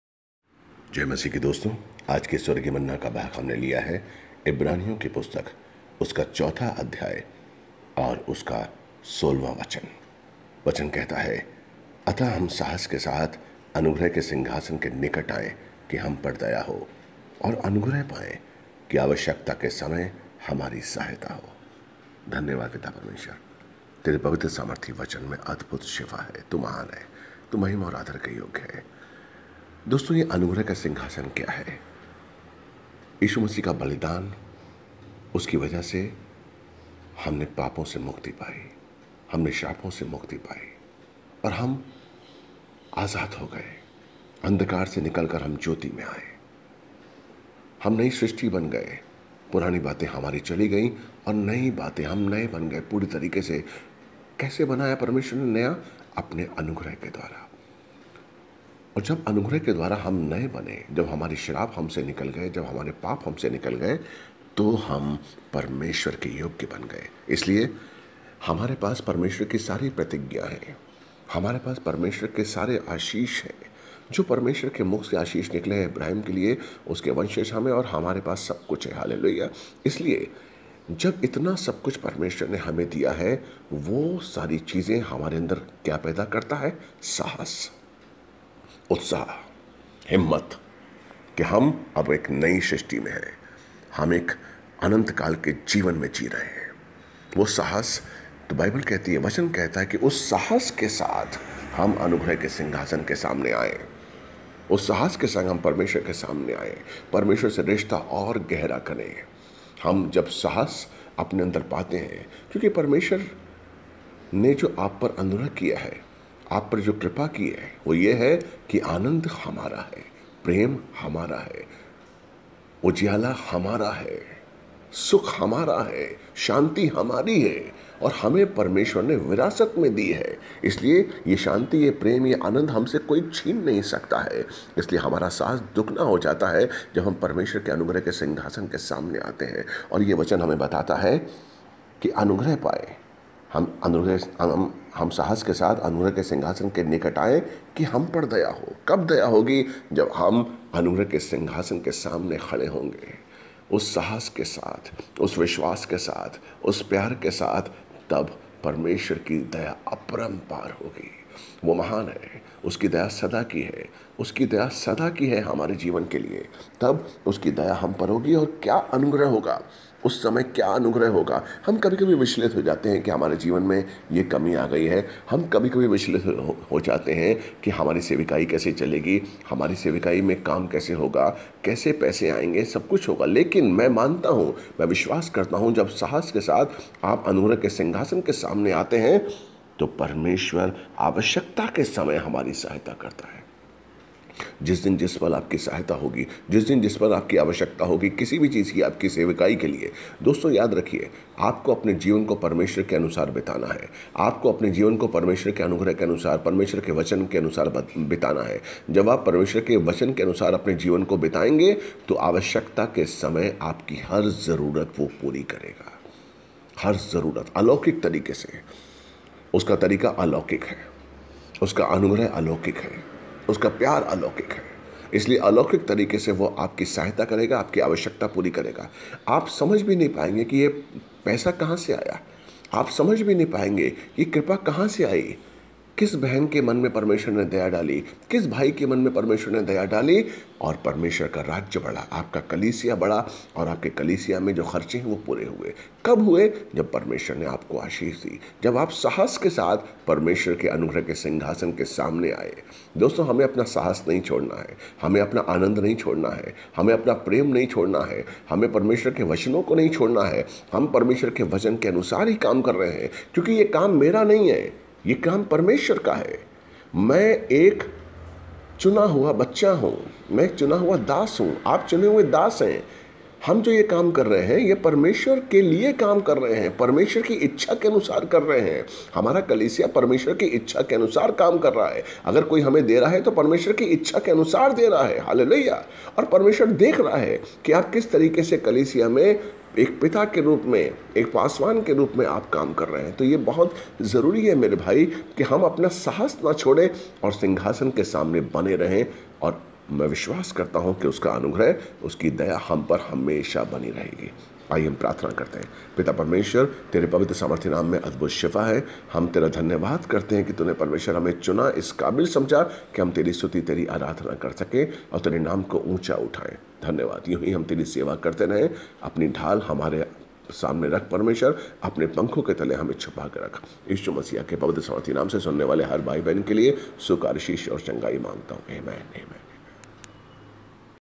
342 Heavenly Manna - Saahas Hamari Punji (Pastor Anil Kochar)
३४२ स्वर्गीय मन्ना - साहस हमारी पूंजी।

इब्रानियों ४:१६
अतः हम साहस के साथ अनुग्रह के सिंहासन के निकट आयें कि हम पर दया हो, और अनुग्रह पाएं, कि आवश्यकता के समय हमारी सहायता हो।

स्वर्गीय मन्ना - पास्टर अनिल कोचर